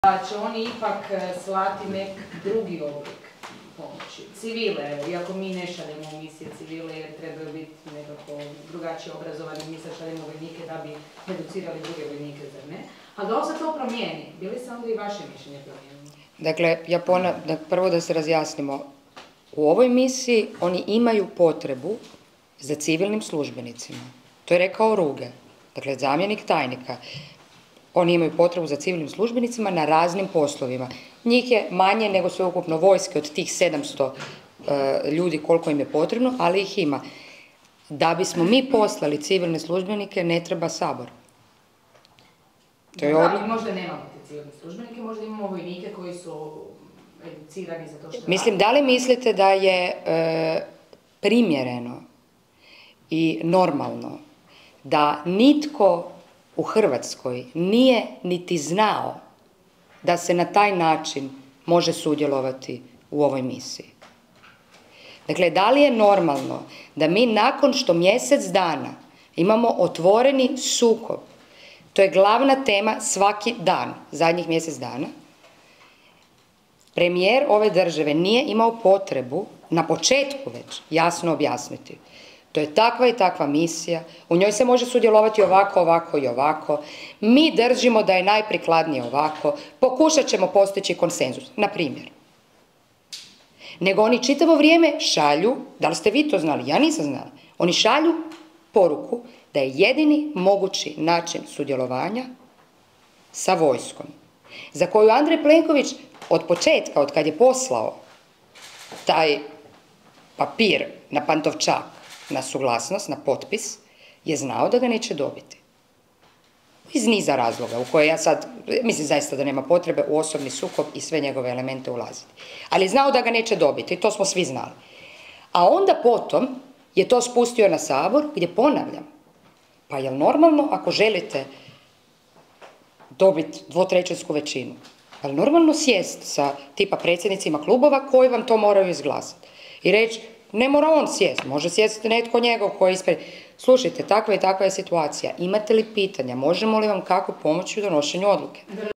Kekin: "Plenković cijelo vrijeme govori da je jedini način sudjelovanja u misiji s vojskom"
Na konferenciji za medije održanoj u sjedištu stranke Možemo!  Ivana Kekin i Sandra Benčić, komentirale su najnovija saznanja o misiji NSATU.